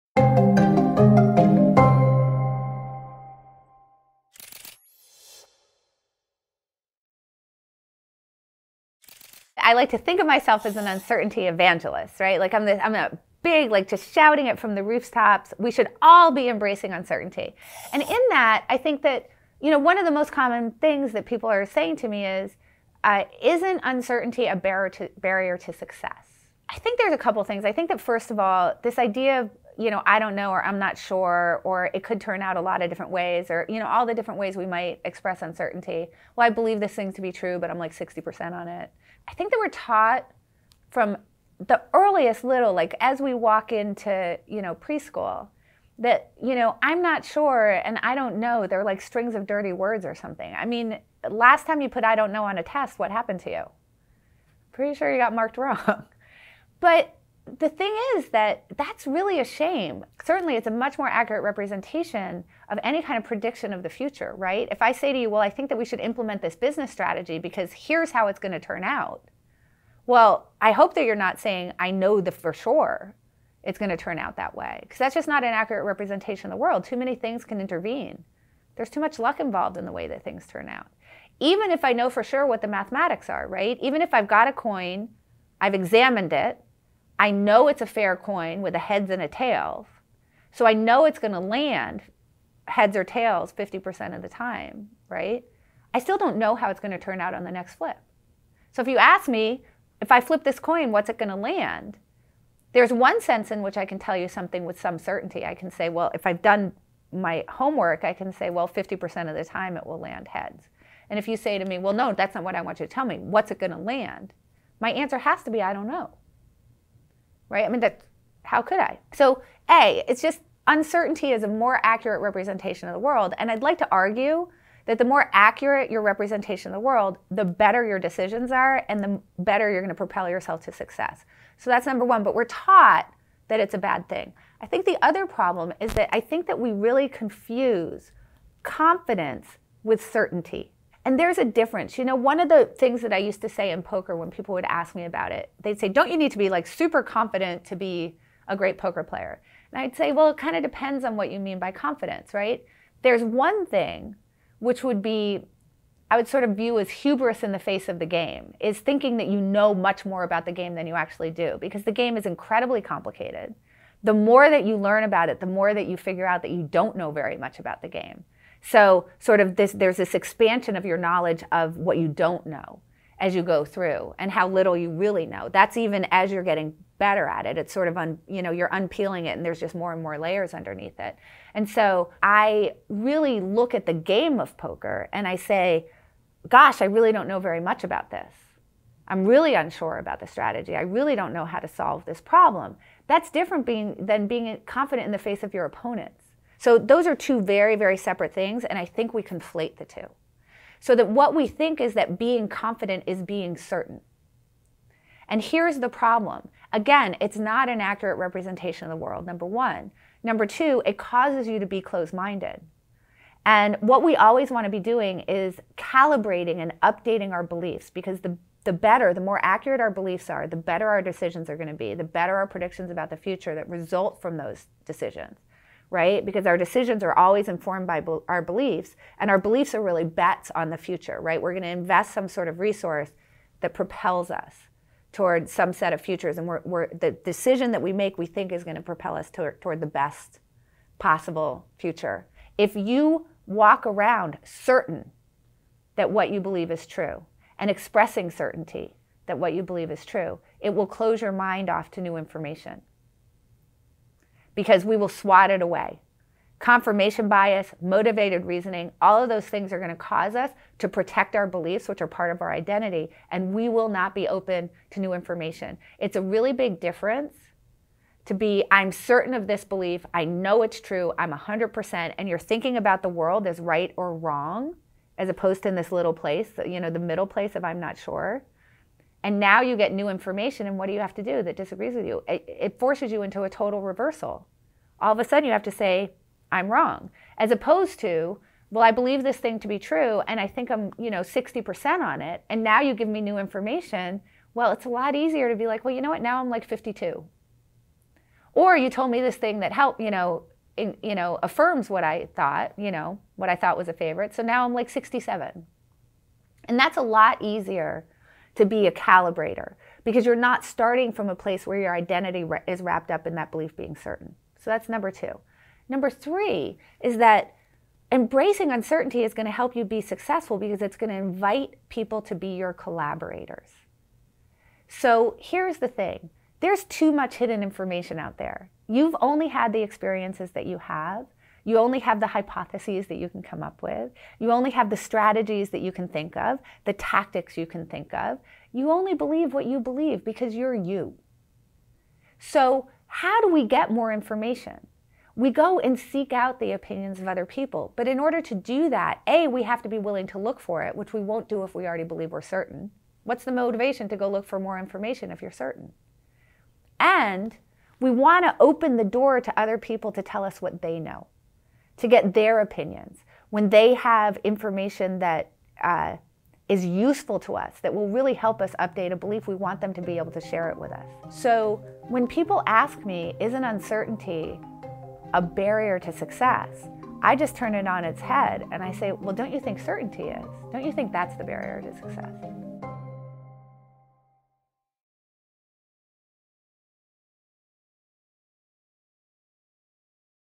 Why being uncertain is a hidden strength | Annie Duke
We’re often ashamed to say  “I don’t know” — but this professional poker player thinks it’s one of our greatest strengths:

From a young age, society teaches us to avoid expressing uncertainty: saying “I don’t know” is a hallmark of failure and shame throughout our formative school years. But former professional poker player Annie Duke contends that admitting uncertainty reflects a more accurate understanding of reality.

While being certain may provide a false sense of security, it can close our minds to new information, hinder the fair calibration of our beliefs, and inhibit fruitful collaboration. Duke also highlights the crucial difference between confidence and certainty: while the former can be beneficial in specific contexts, like facing an opponent in poker, the latter can lead to overconfidence and hubris.

Duke argues that acknowledging uncertainty invites collaboration, as individuals actively seek out diverse opinions to form a comprehensive understanding. In contrast, certainty can limit learning and growth, potentially becoming a real obstacle to success.